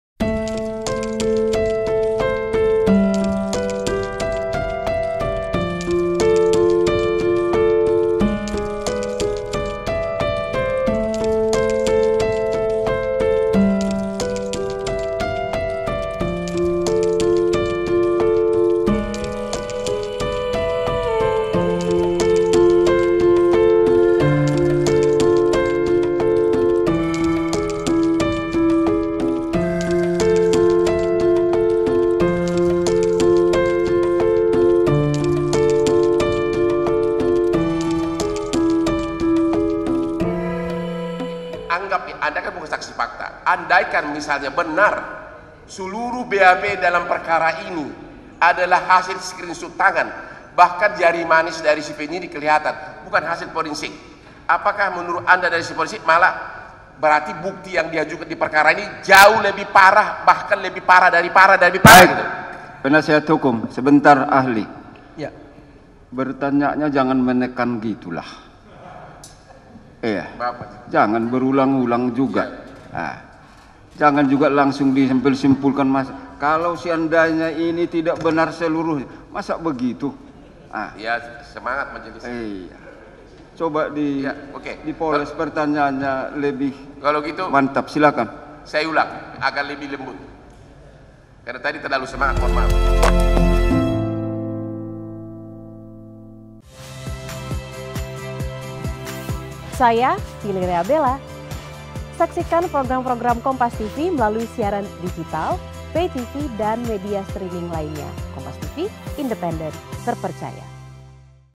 Ditegur Hakim, Hotman Paris Cengengesan Minta Maaf
JAKARTA, KOMPAS.TV - Penasihat hukum Teddy Minahasa, Hotman Paris Hutapea minta maaf ke majelis hakim di sidang kasus narkoba di Pengadilan Negeri (PN) Jakarta Barat, Senin (13/3/2023).

Pasalnya, Hotman dapat teguran keras dari ketua majelis hakim, Jon Sarman Saragih lantaran mencecar saksi ahli digital forensik.

"Jauh lebih parah, bahkan lebih parah, lebih parah?" Tanya Hotman.

"Penasihat hukum, sebentar ahli. Bertanyanya jangan menekan gitu, jangan berulang-ulang juga, jangan juga disimpulkan, kalau seandainya ini tidak benar seluruhnya, masa begitu," kata ketua hakim.

"Maaf majelis," ujar Hotman sambil sikap hormat.

"Coba dipoles pertanyaannya agar lebih mantap, silakan," kata hakim mengingatkan.

"Saya ulang agar lebih lembut, karena tadi terlalu semangat mohon maaf," ujar Hotman.

Baca Juga Keras! Teddy Minahasa Keberatan Jaksa Salah Simpulkan Fakta: Mungkin Beliau Merangkap Hakim di